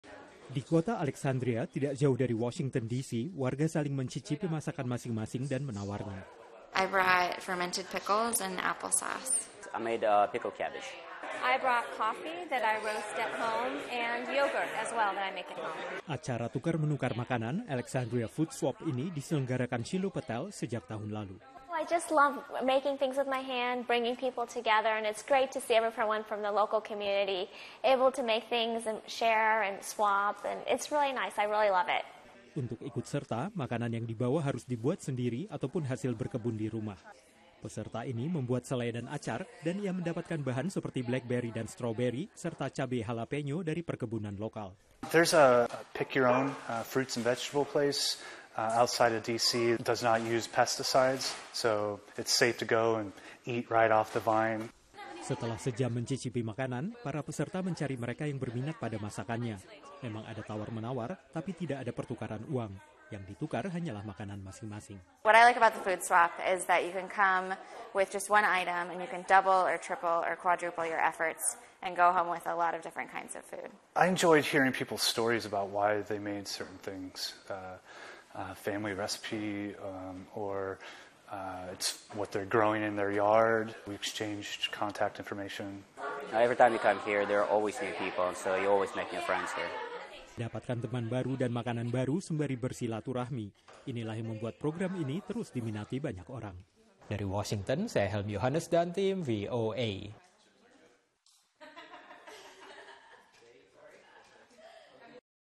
Barter Makanan di Amerika - Liputan Feature VOA
Sejumlah komunitas amerika ada yang menerapkan sistem barter atau pertukaranan makanan. Pertukaran makanan yang dimulai oleh warga Brooklyn, New York ini menyebar di kota lain dengan bantuan internet. Selengkapnya ikuti laporan tim VOA berikut ini.